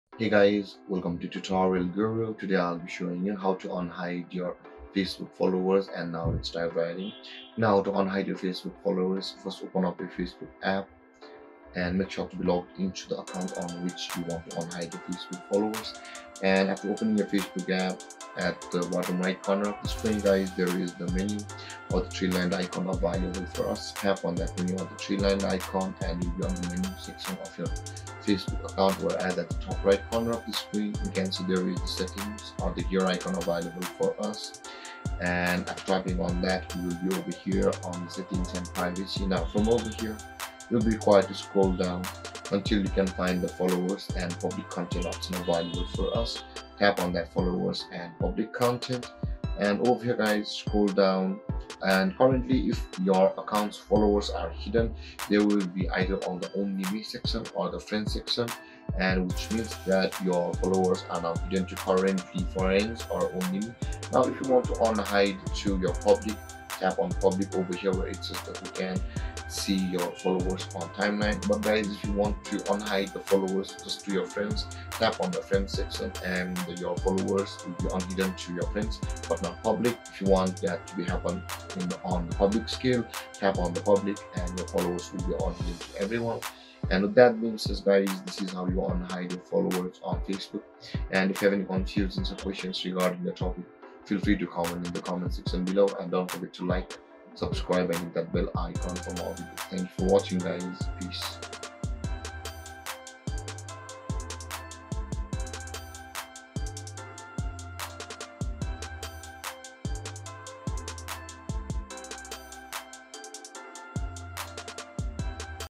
How to Unhide Followers on Facebook?
Do you want to unhide your followers on Facebook? In this video, I will show you how to do it in a few easy steps.

By default, your Facebook followers are hidden from you. This means that you can't see who is following you and you can't see how many followers you have. However, you can choose to unhide your followers if you want.